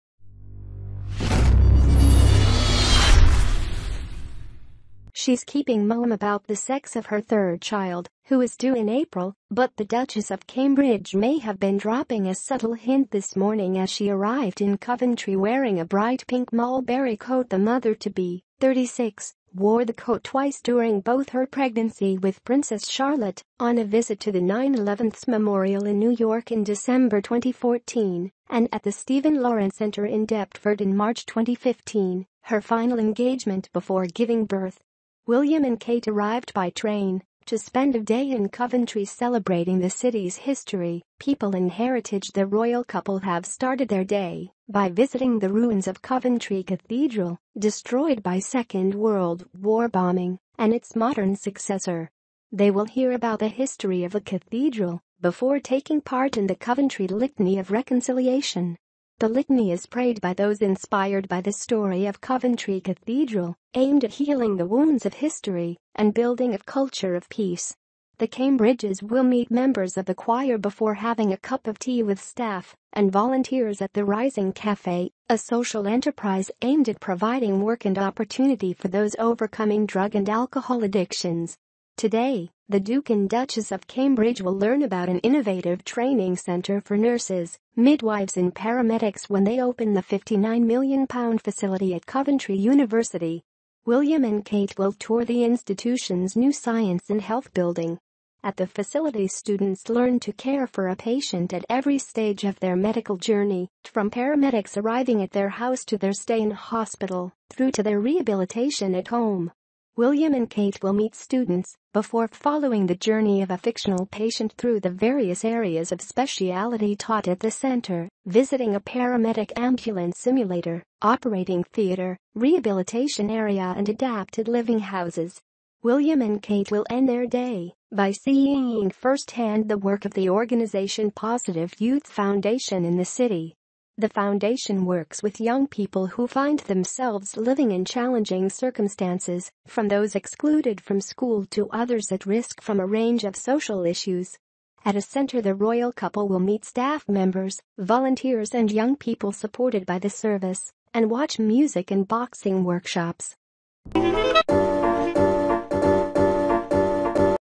Pregnant Kate appears sparkling in a cerise Mulberry coat on a go to to Coventry wit The Royal News
The Royal News Pregnant Kate appears glowing in a cerise Mulberry coat on a go to to Coventry with Prince William
She's maintaining mum about the sex of her 0.33 toddler, who is due in April, but the Duchess of Cambridge may additionally have been losing a diffused hint this morning as she arrived in Coventry sporting a vibrant pink Mulberry coat
The mom-to-be, 36, wore the coat two times at some point of each her pregnancy with Princess Charlotte - on a go to to the 9-11 memorial in New York in December 2014  and on the Stephen Lawrence Centre in Deptford in March 2015, her final engagement before giving birth.
Thank you for seeing our movies
My weblog link :
My fb hyperlink :